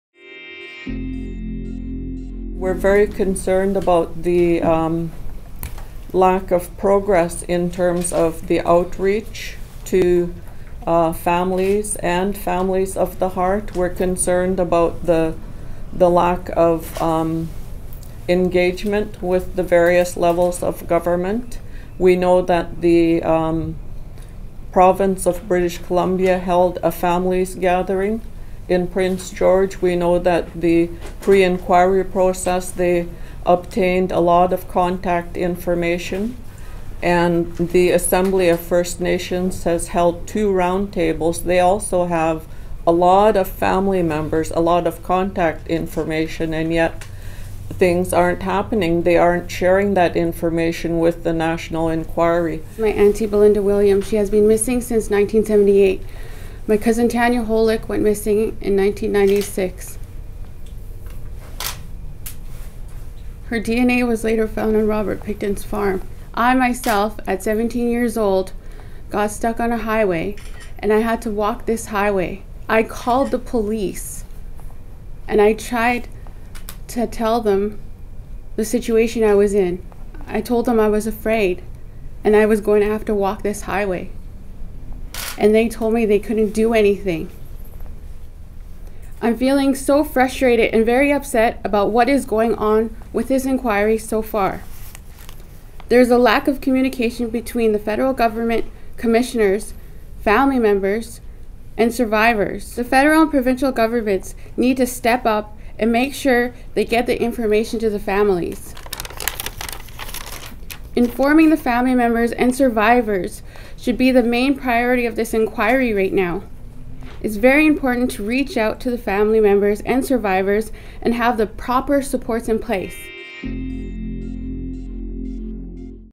Missing women inquiry not reaching out to families: Advocates
A coalition of B.C. advocacy groups is criticizing the national inquiry into missing and murdered indigenous women. Coalition member Fay Blaney says there hasn’t been enough outreach to the affected families. (April 3)